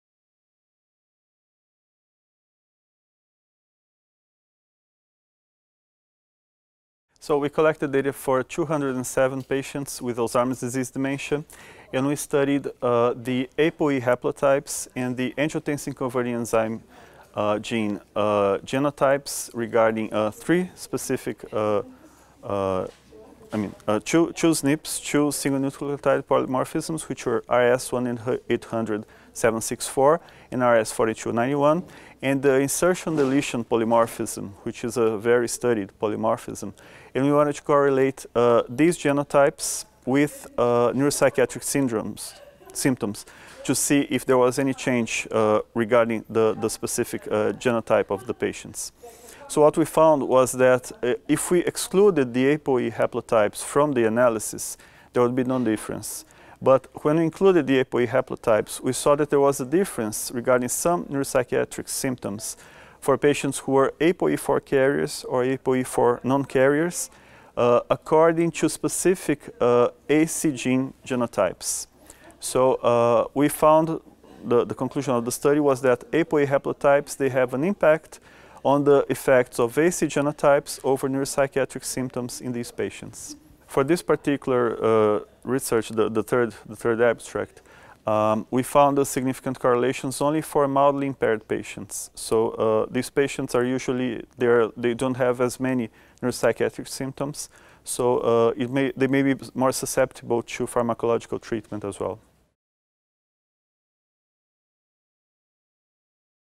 APOE haplotypes, ACE insertion/deletion polymorphism and neuropsychiatric symptoms in Alzheimer’s
Fabricio Ferreira De Oliveira, MD, PhD from the Federal University of São Paulo, São Paulo, Brazil gives an overview of a study on the APOE haplotypes and the angiotensin-converting enzyme (ACE) insertion/deletion polymorphism and their association with neuropsychiatric symptoms in Alzheimer’s disease.
Recorded at the 2017 Congress of the European Academy of Neurology (EAN) in Amsterdam, the Netherlands.